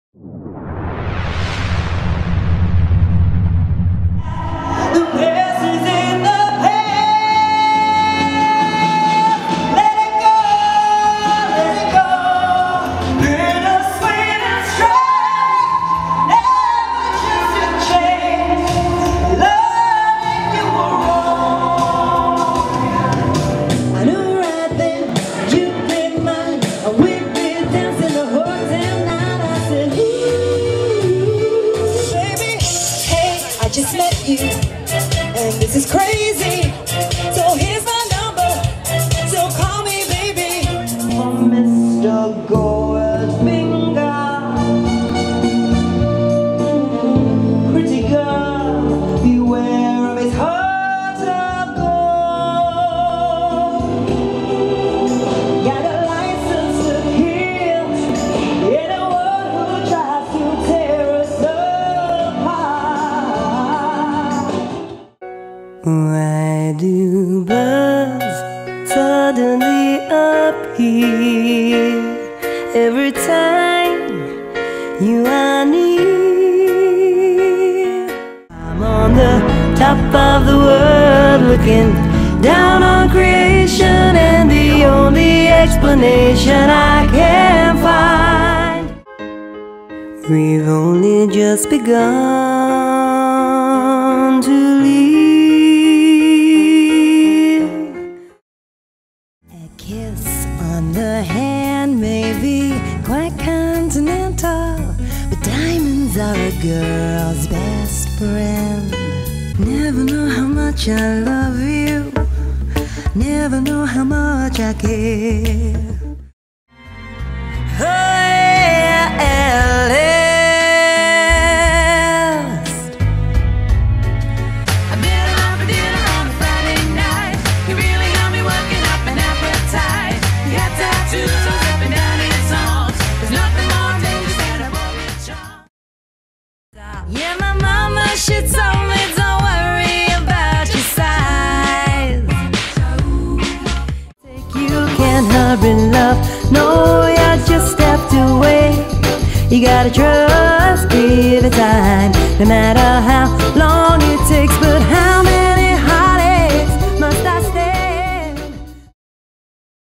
Hollie Kamel Showreel 2016
Hollie Kamel professional Singer specialising in Themed Shows and Party Sets perfect for all Special Occasions & Events.

Shows include:
Bond (007)
Gatsby
Westend, Broadway & Disney
Best of Country
Best of 80s
Vintage/Jazz Show
Thought the Decades Party Show
Celebrating the Carpenters